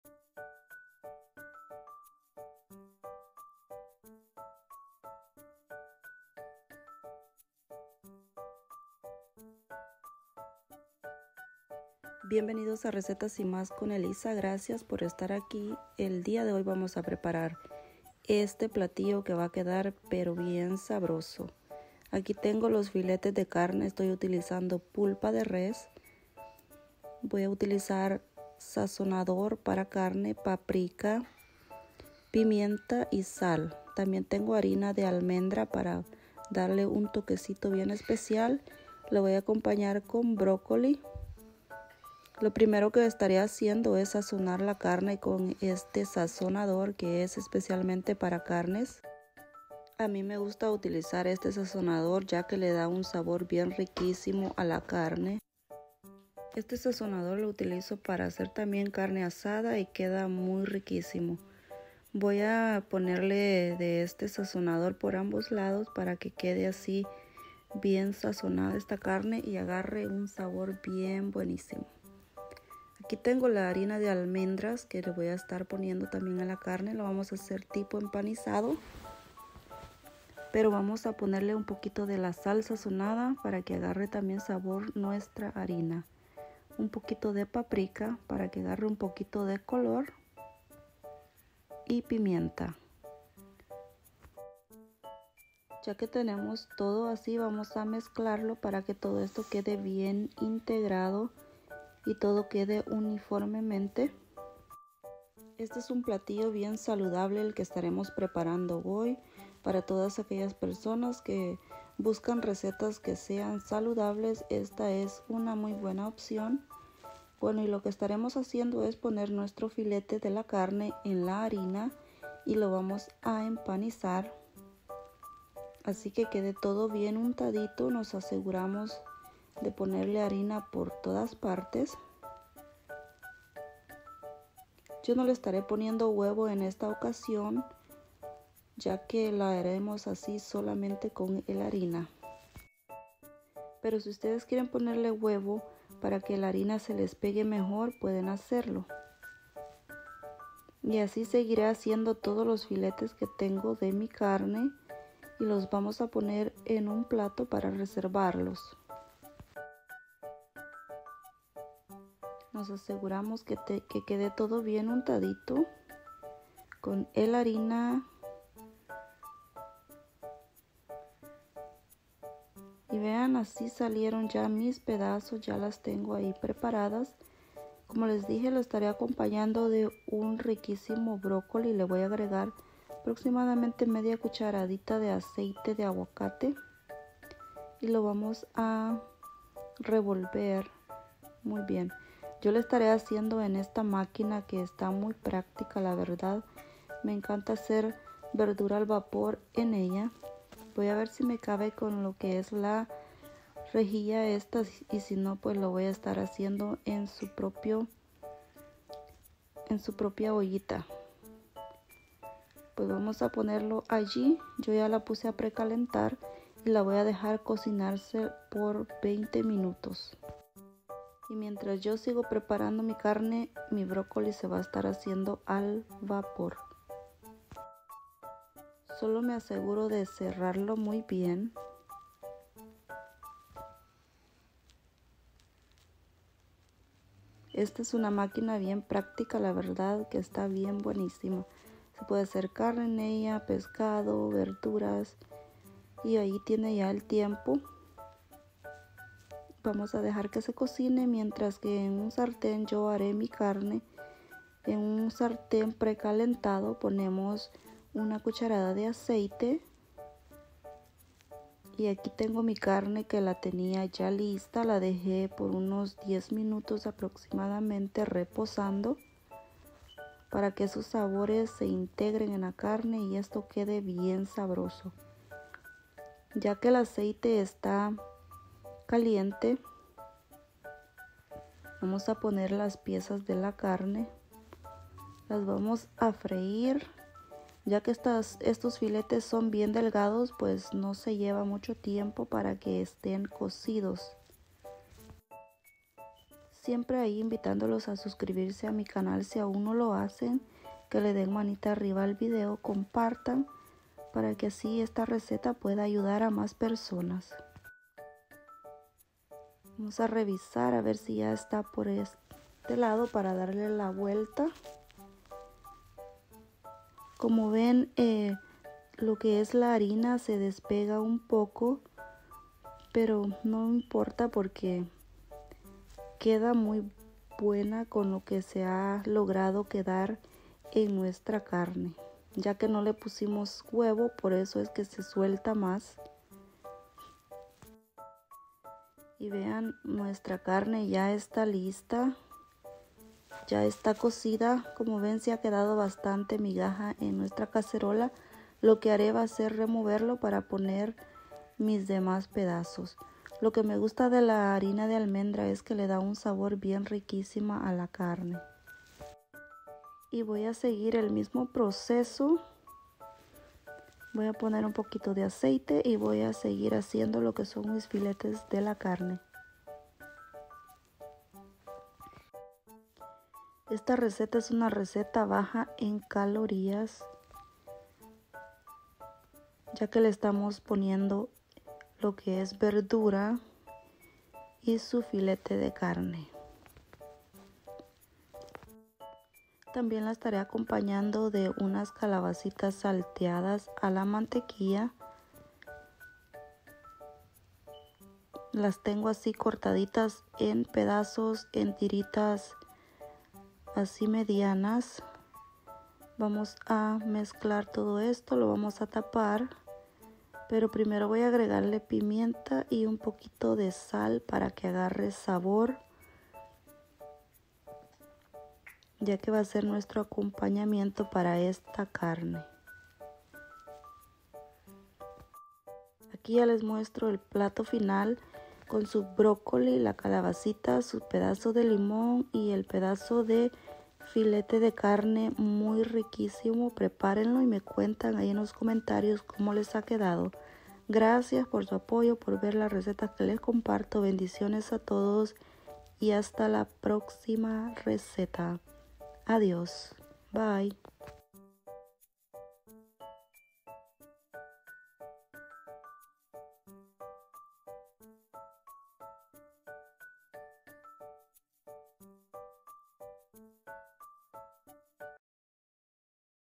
Una Receta saludable filete de carne con brócoli y calabaza salteada
Quieres comer sano , saludable y riquísimo claro que si aquí te dejo esta receta sabrosa fácil y rápida de preparar
Filete de carne con brócoli y calabaza
Una receta 2x1 bien buena